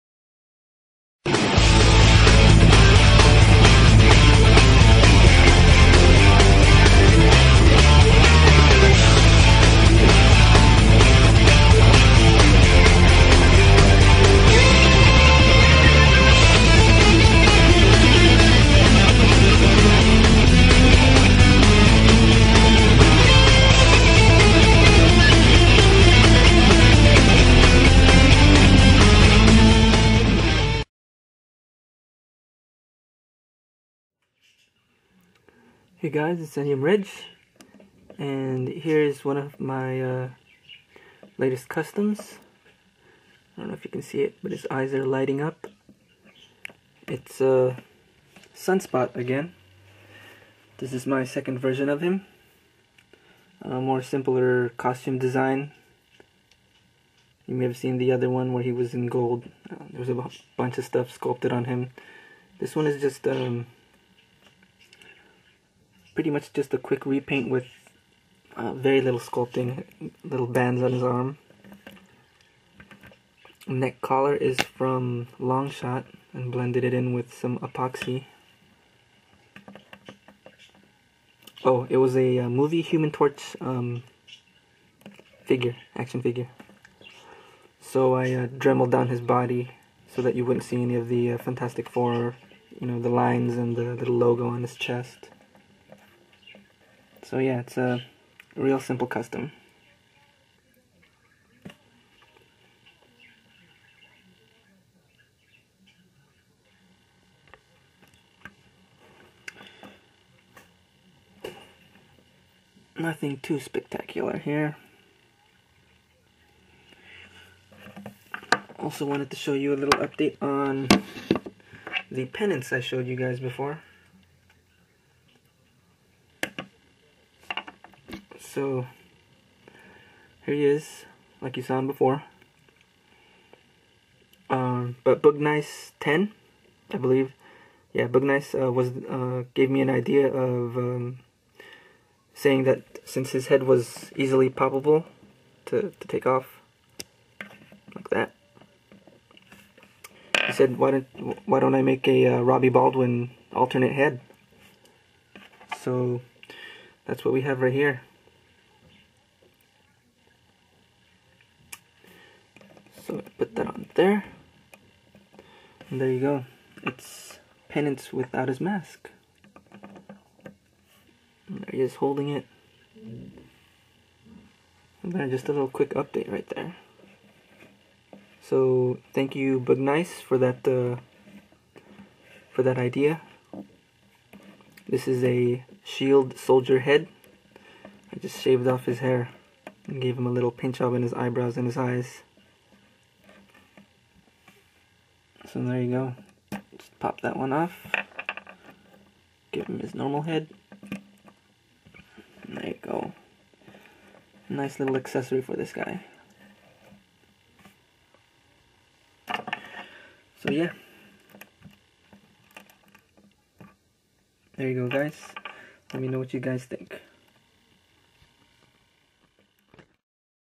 Custom Marvel Legends: Sunspot V.2
My 2nd version of Sunspot! and a variant head for Penance!

for the variant Robbie Baldwin head idea!